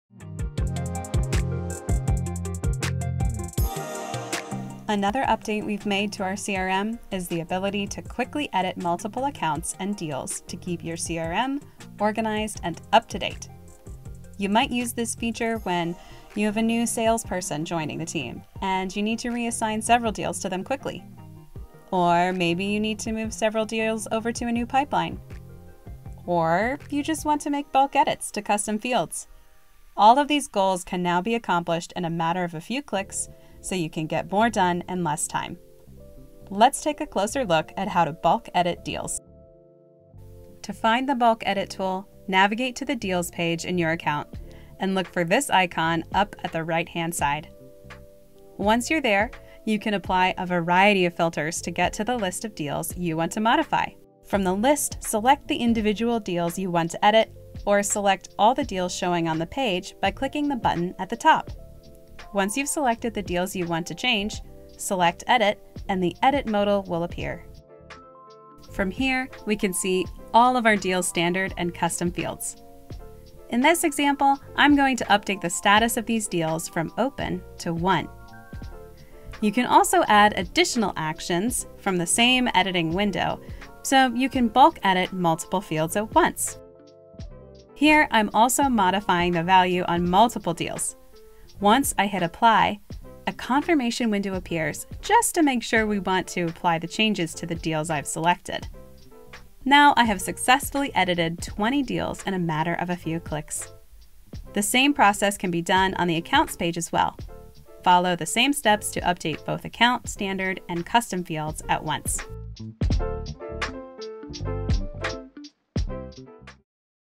Bulk Edit Multiple Accounts and Deals in the CRM
Quickly edit multiple accounts to keep your CRM organized and up-to-date with bulk edit enhancement. Update multiple account’s standard and custom fields to boost your team’s productivity.